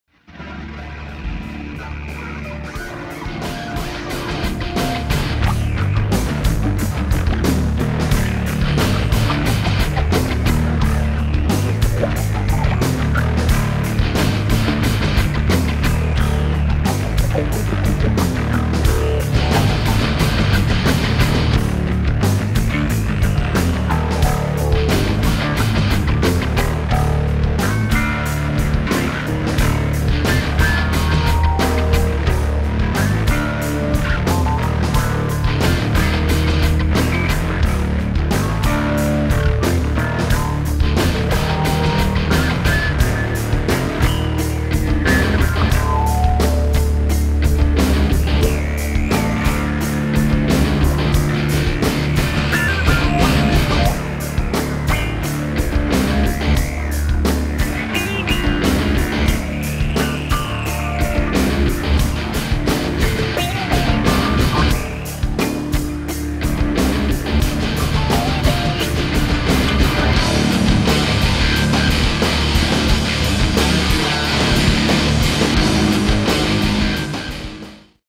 the destroyer monster truck photos
a timeline video  of the destroyer the driver's are Dan Evans and Roger Stidell it used to be Dan Evans but Roger purchased the truck in December 2011 but now here is destroyer today